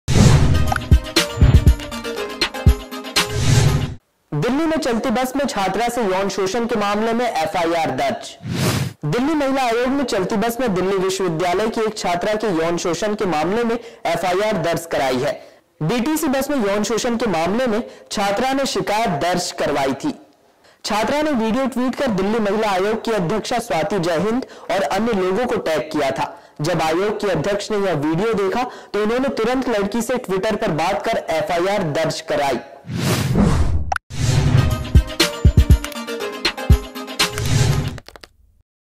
दिल्ली में चलती बस में छात्रा से यौन शोषण मामले में FIR दर्ज
दिल्ली महिला आयोग ने चलती बस में दिल्ली विश्वविद्यालय की एक छात्रा के यौन शोषण के मामले में एफआईआर दर्ज कराई है, डीटीसी बस में यौन शोषण के मामले में छात्रा ने शिकायत दर्ज कराई थी. छात्रा ने वीडियो ट्वीट कर दिल्ली महिला आयोग की अध्यक्षा स्वाति जय हिन्द और अन्य लोगों को टैग किया था. जब आयोग की अध्यक्ष ने वह वीडियो देखा तो उन्होंने तुरंत लड़की से ट्विटर पर बात कर FIR दर्ज कराई.